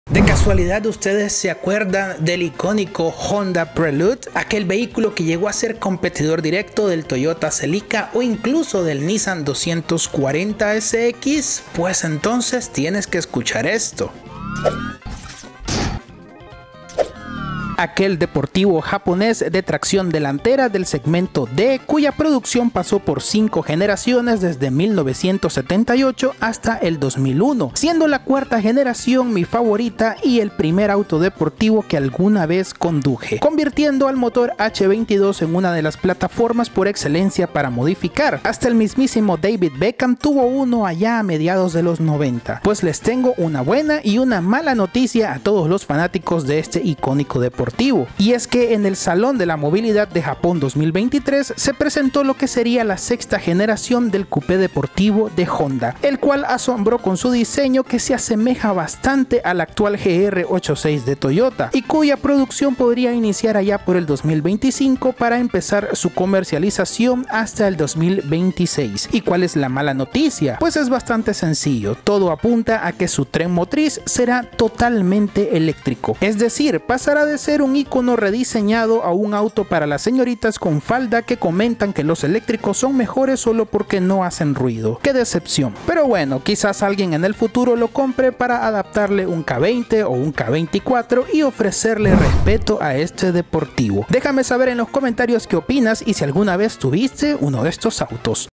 Honda Prelude 2024.
Honda anuncia el próximo relanzamiento del Honda Prelude continuando con la 6ta generación del deportivo de la marca.